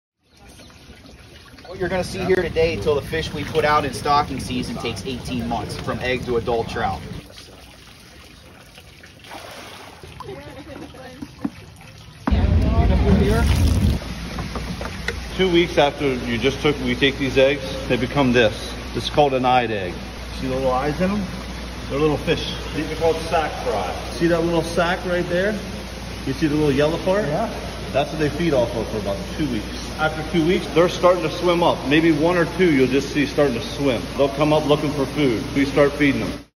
Spawning rainbow trout at Huntsdale State Fish Hatchery
▪️ 18 months from egg to adult trout
▪️ 3,000 eggs per female
▪️ 500,000 fish produced annually